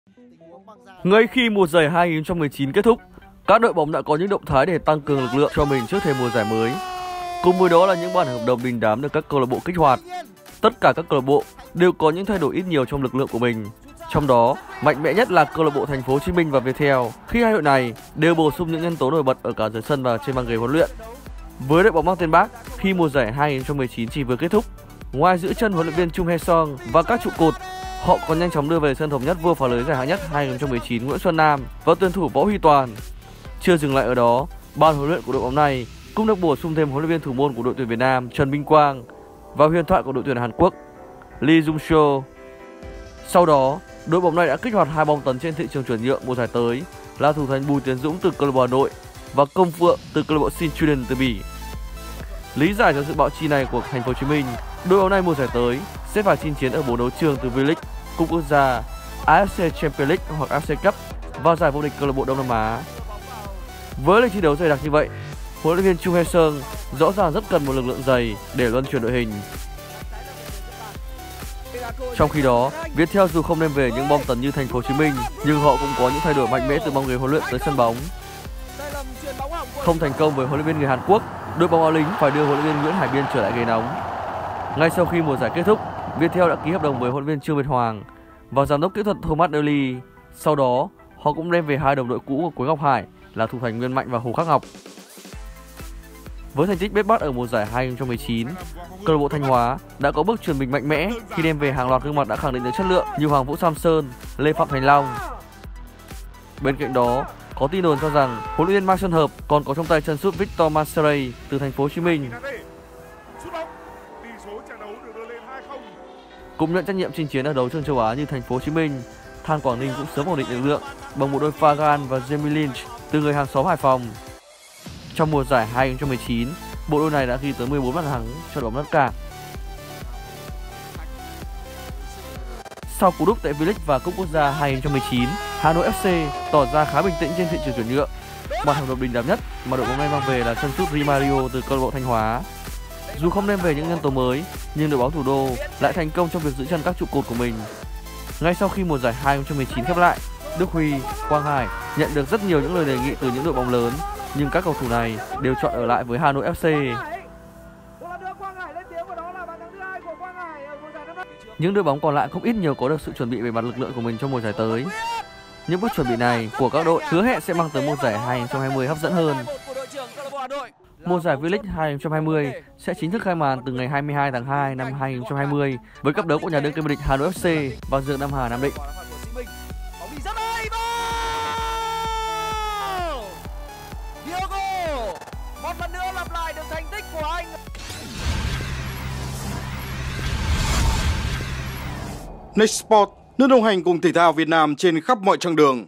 Bùi Tiến Dũng, Công Phượng và những thương vụ đình đám nhất trước thềm V.League 2020 | NEXT SPORTS
NEXT SPORTS | Bùi Tiến Dũng, Công Phượng và những thương vụ đình đám nhất trước thềm V.League 2020

Ngay khi mùa giải 2019 kết thúc, các đội bóng đã có những động thái để tăng cường lực lượng cho mình trước thềm mùa giải mới. Cùng với đó là những bản hợp đồng đình đám được các CLB kích hoạt.